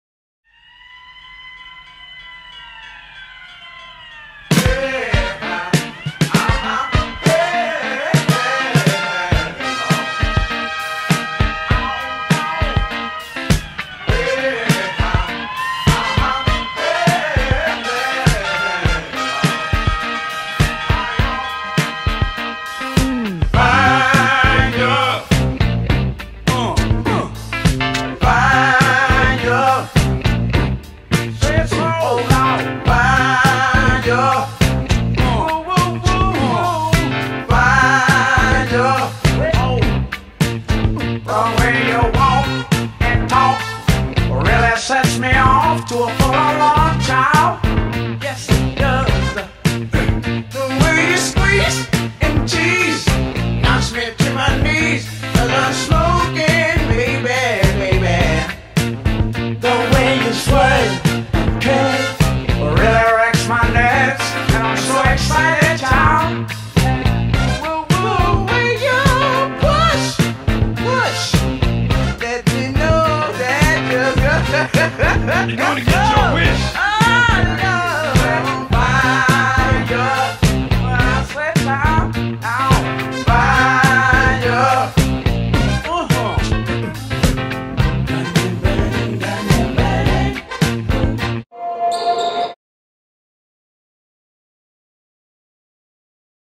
Stuyvesant Phoenix girls basketball v Fashion Industries 12-18-13
Stuyvesant Phoenix easily dispose of the HS of Fashion Industries. Video shows some of Sophie Gershon's game high 22 points. Lauren Sobota with some nice dishes.